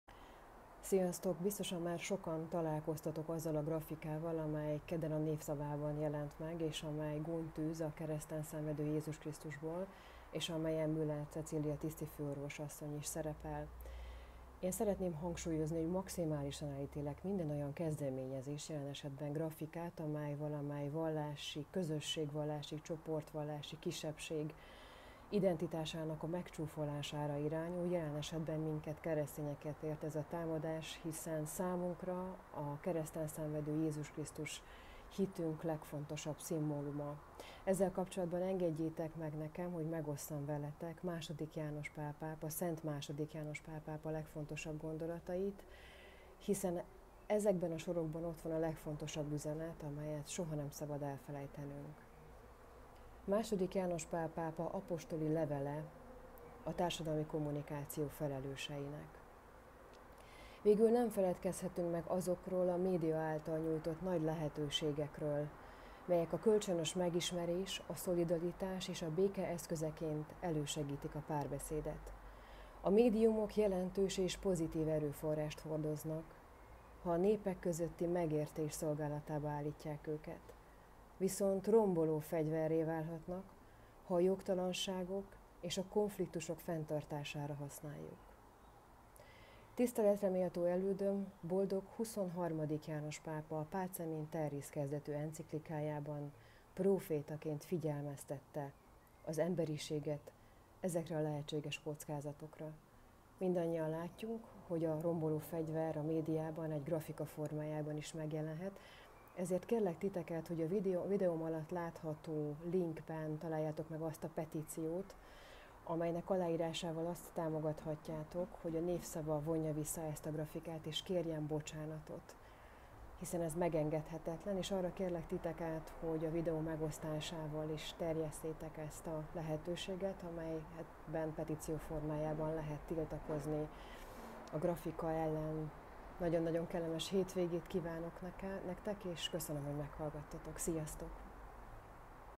Senkinek nincs joga gúnyt űzni Jézus Krisztusból!
Elég volt a baloldali sajtó vallásgyalázó gyakorlatából!‼️
Írd alá a petíciót itt 👇✝️